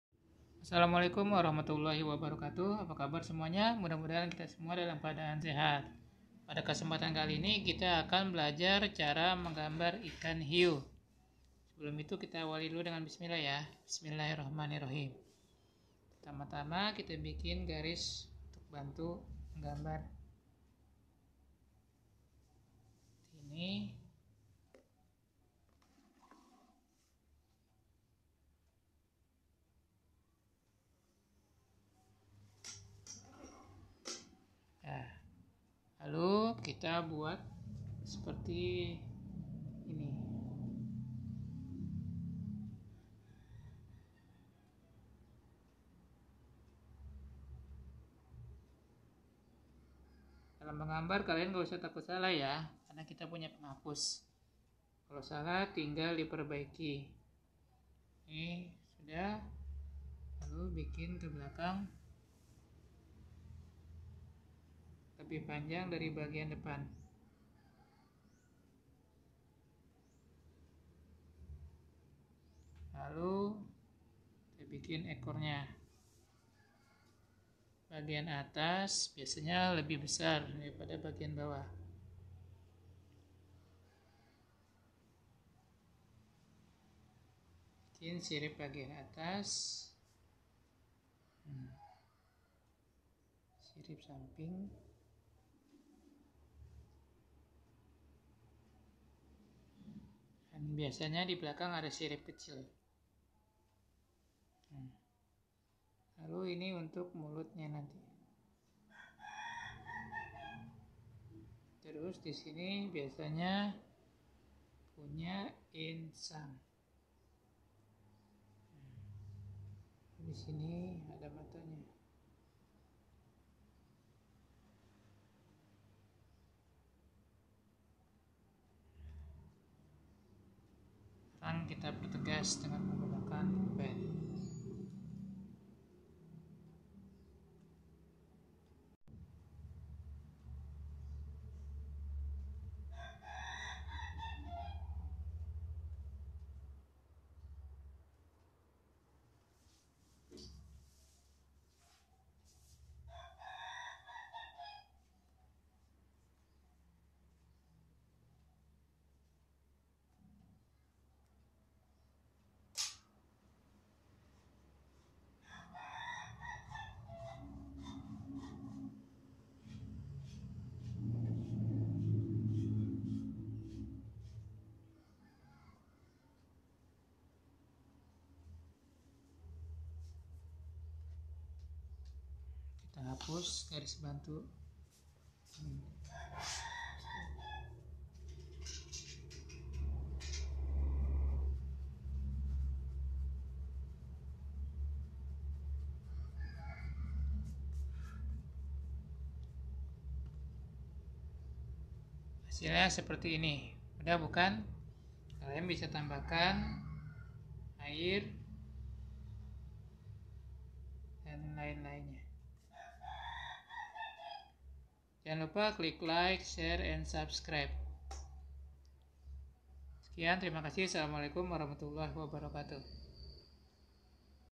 Cara menggambar ikan hiu. How to draw shark
Apa kabar semuanya. Mudah2an kita semua semua dlm keadaan sehat dan dalam kasih sayang Allah SWT.Aamiin.

Pada kesempatan kali ini, kita akan belajar menggambar ikan hiu. Mudah-mudahan bermanfaat. Aamiin

Yg mau request cara menggambar, mengerjakan matematika, atau ada pertanyaan, tulis d kolom komentar ya.
Wassalamualaikum

xxxxxxxxxxxxxxxxxxxxxxxxxxxxxxxx
Lihat gambaran lain yang saya buat di